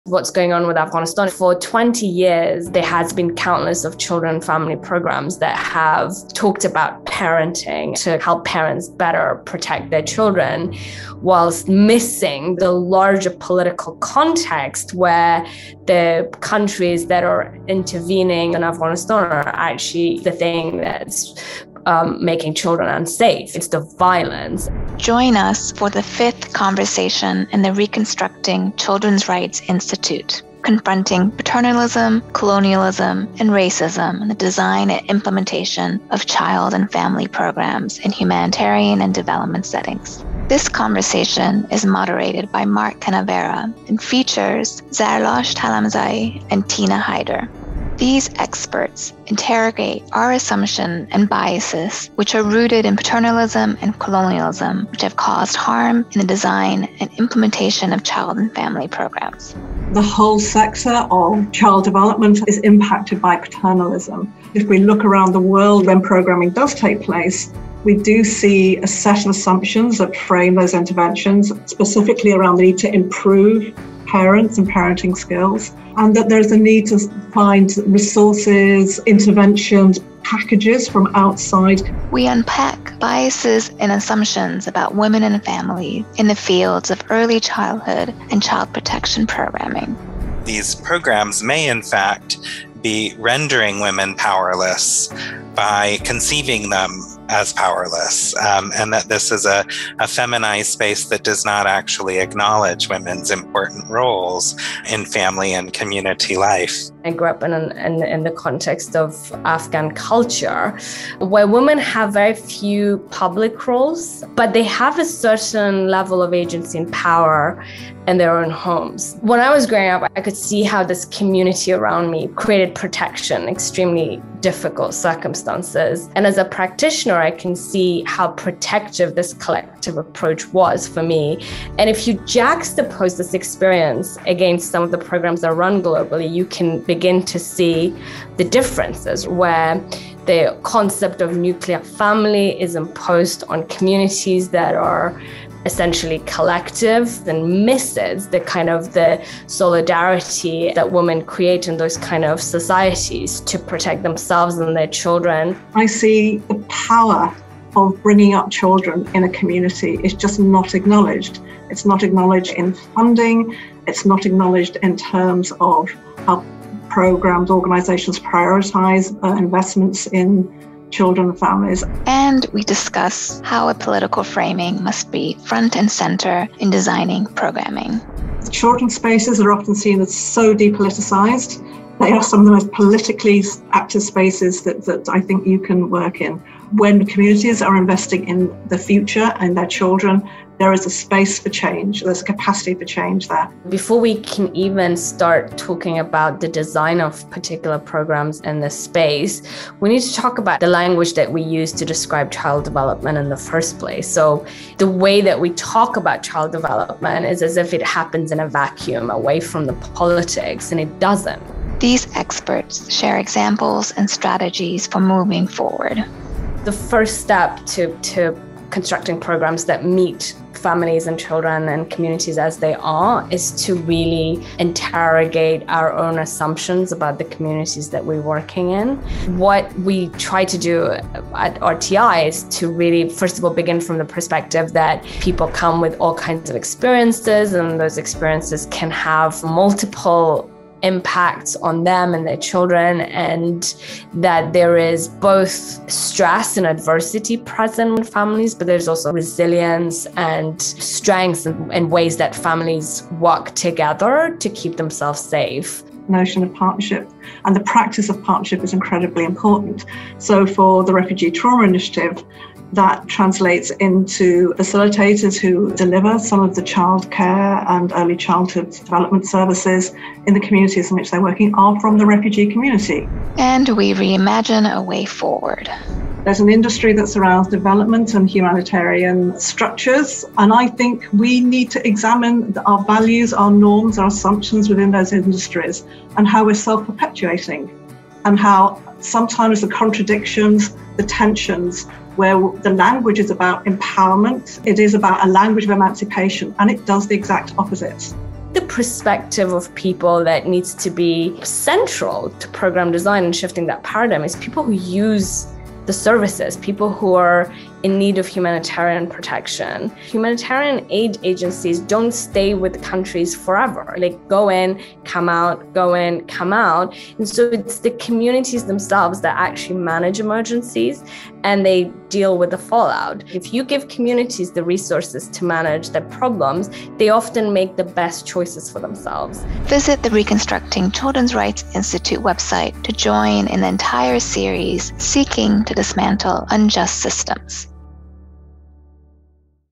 Trailer: Conversation #5, Design and Implementation of Child and Family Programs
We are excited to launch the Reconstructing Children's Rights Institute, an online institute about dismantling racism, neo-colonialism, and patriarchy in humanitarian and development efforts to protect children and support families.

The fifth conversation in this series, Confronting Paternalism, Neo-Colonialism, and Racism in the Design and Implementation of Child and Family Programs in Humanitarian and Development Settings, will take a critical look at the ways that humanitarian and development actors design and implement child development and family support programs, especially ways in which paternalistic, neocolonial, and racist thinking can imbue such programs. We are joined by two experts – Zarlasht Halaimzai and Tina Hyder.  The conversation will be global in nature but will also feature a timely, unfortunate focus on Afghanistan.

The full conversation and accompanying briefing note will be released on November 22, 2021.